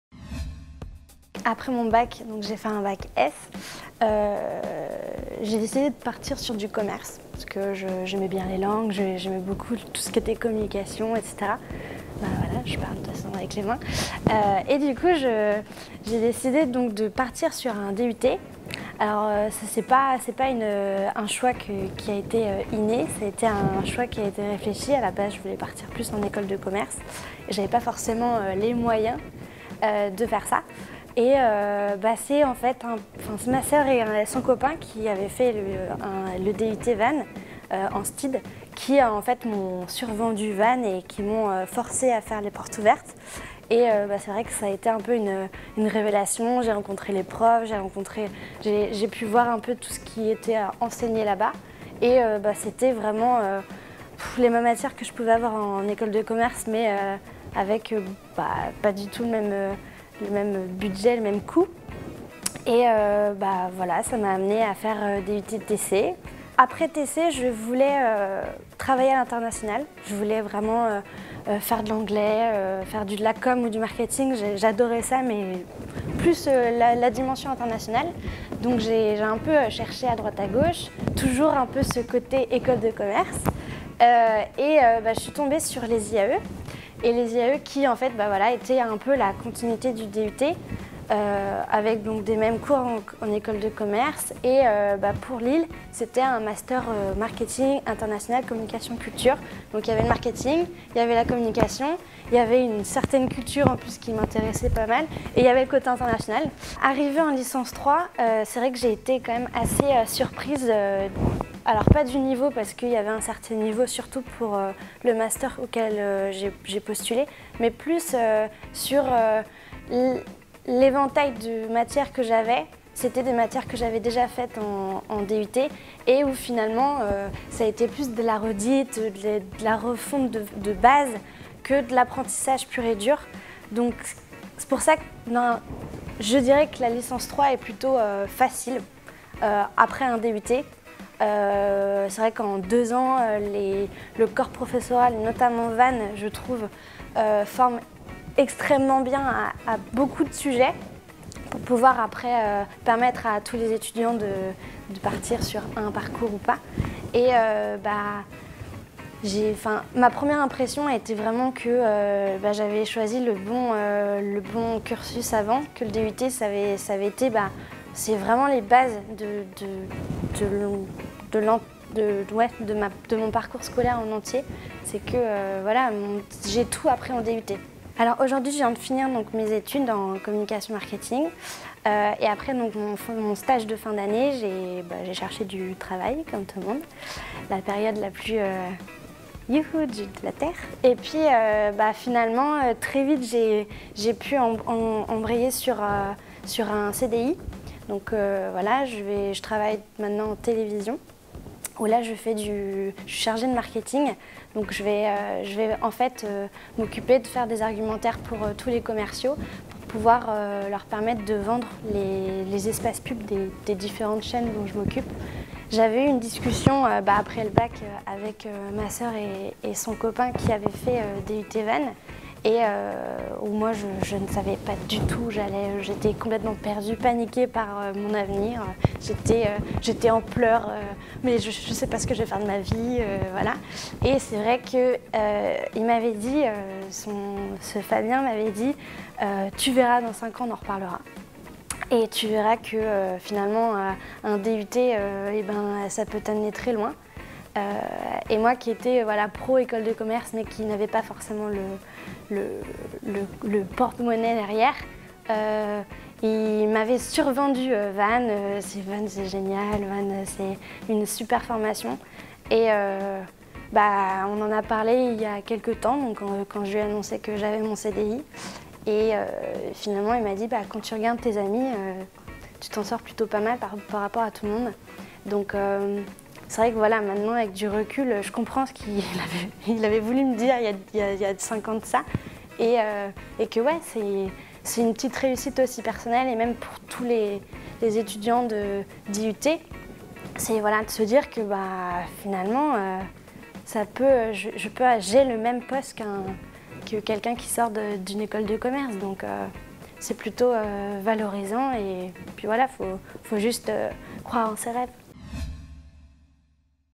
IUT Vannes - Témoignage d'une diplômée DUT TC Promo 2012
Pauline Castello, est diplômée d'un DUT Techniques de Commercialisation promotion 2012. Elle a continué ses études à l'IAE de Lille. Elle est maintenant dans la vie active et exerce en qualité d'assistante chargée d'études marketing chez TF1 Publicité. Un très beau parcours à découvrir !